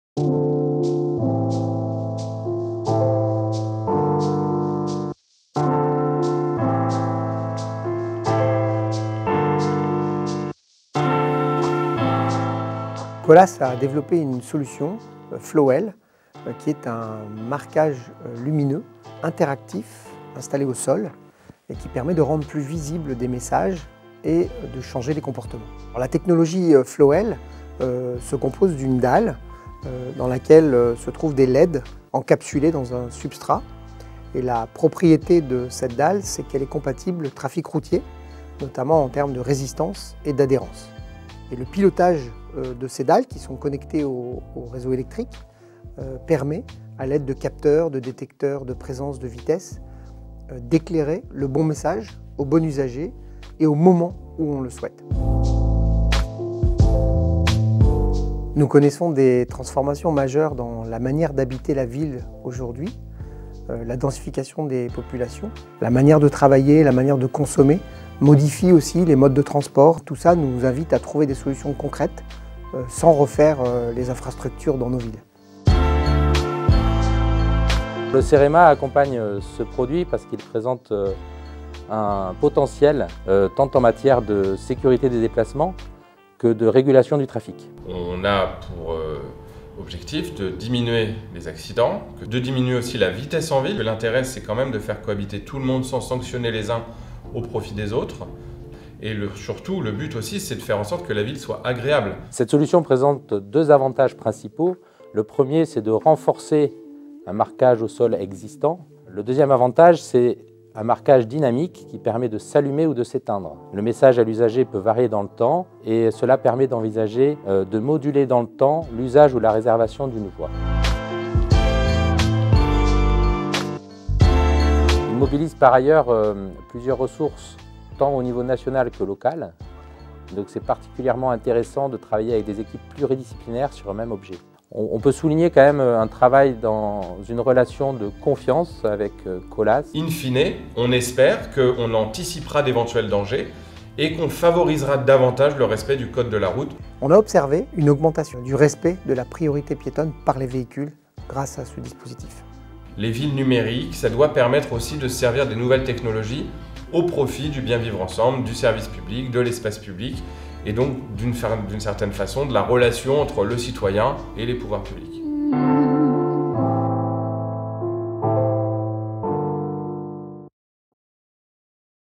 Flowell - marquage lumineux interactif au service des villes et de ses usagers
Flowell est un marquage lumineux interactif et dynamique permettant d’envoyer des messages aux usagers de la route. Cette solution vise à anticiper les dangers et à favoriser le respect du code de la route.
🎥 Satellite Multimedia